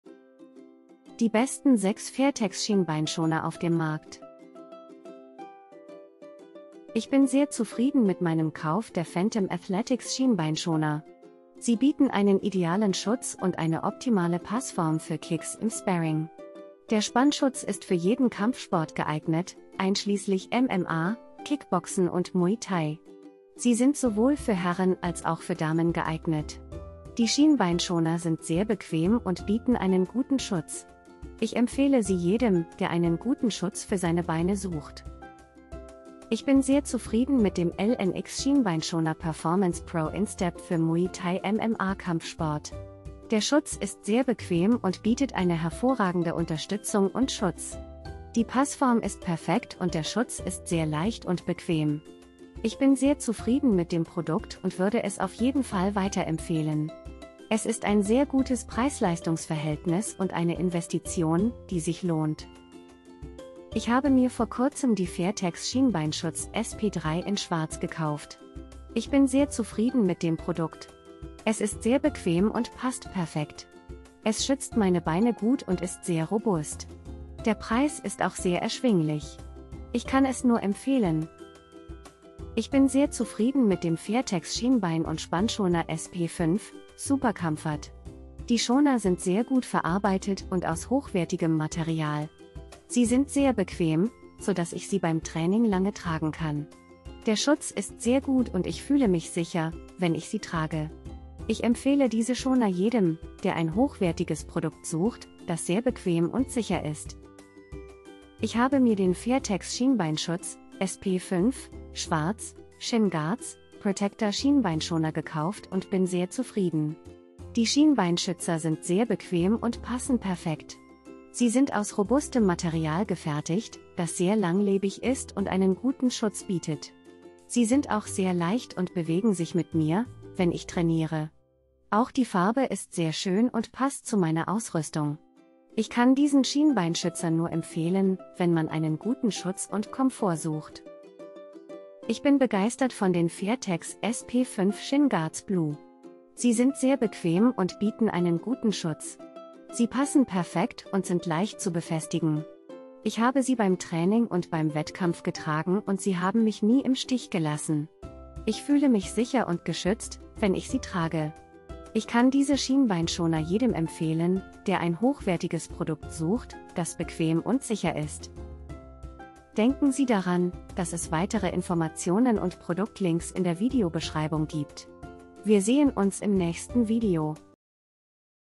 FAIRTEX-SCHIENBEINSCHONER: Die besten 6 Fairtex-Schienbeinschoner auf dem Markt! 🙂
✅ Alle Produkte

✋  Hier sind die besten 6 fairtex-schienbeinschoner auf dem markt! auf dem Markt.

🔽🔽🔽 KAUFEN-LINKS FAIRTEX-SCHIENBEINSCHONER 🔽🔽🔽

1) Phantom Athletics Schienbeinschoner - Idealer Schutz und Optimale Passform für Kicks im Sparring - Mit Spannschutz für jeden Kampfsport - MMA, Kickboxen und Muay Thai - Herren und Damen

2) LNX Schienbeinschoner "Performance Pro" Instep für Muay Thai MMA Kampfsport

3) Fairtex Schienbeinschutz, SP3, schwarz, Schienbeinschoner, Shin Pads, Guards MMA

4) Fairtex Schienbein- und Spannschoner SP5 - Super Comfort

5) Fairtex Schienbeinschutz, SP5, schwarz, Shin Guards, Protector Schienbeinschoner

6) Fairtex SP5 Shin Guards Blue

* Zeitindikatoren (Timestamps) *

00:00 - Intro
00:05 - 1) Phantom Athletics Schienbeinschoner - Idealer Schutz und...
00:35 - 2) LNX Schienbeinschoner "Performance Pro" Instep für Muay Thai...
01:02 - 3) Fairtex Schienbeinschutz, SP3, schwarz, Schienbeinschoner,...
01:21 - 4) Fairtex Schienbein- und Spannschoner SP5 - Super Comfort...
01:47 - 5) Fairtex Schienbeinschutz, SP5, schwarz, Shin Guards,...
02:20 - 6) Fairtex SP5 Shin Guards Blue...
02:48 - Outro